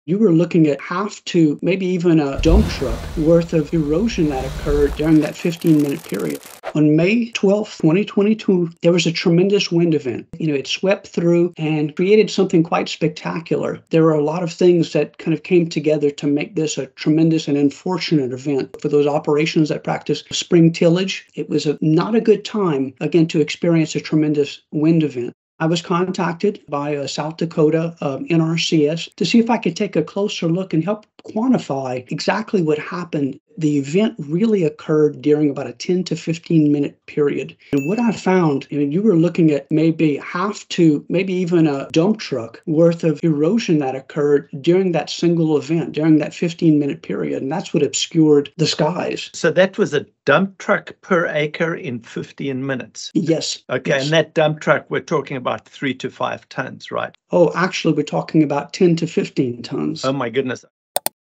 Tons of Soil Per Acre GONE
This short clip is from a podcast interview on The Soil Health Labs podcast between Dr. Buz Kloot (host) and Chris Corriel (National Erosion Specialist).